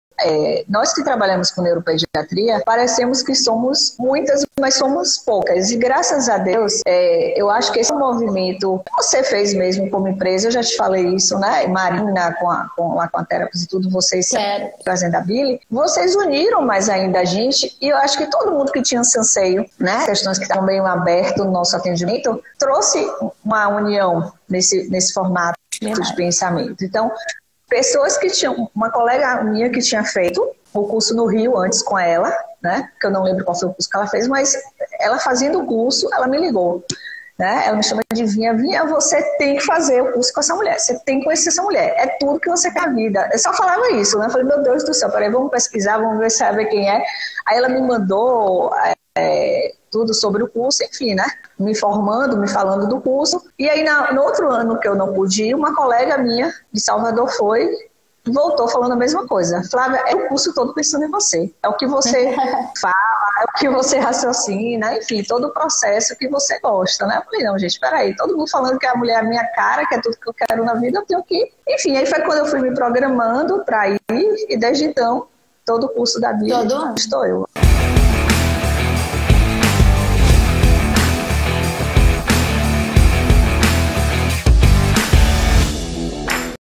TODO CURSO DA BEVERLY CUSICK LÁ ESTOU EU | FISIOVITAL
DESCRITIVO

Juliana Galindo entrevista mais um grande nome da fisioterapia brasileira, a instrutora TheraTogs no Brasil Flávia Filgueiras, e neste vídeo ela explica como conheceu a metodologia da Billi, o TheraTogs e desde então não perde um curso dela no Brasil. Assista o Vídeo e Compartilhe.

PARCEIROS

► TheraTogs
► SureStep
► Fabrifoam
► VitalTape
► Adaptadores de Escrita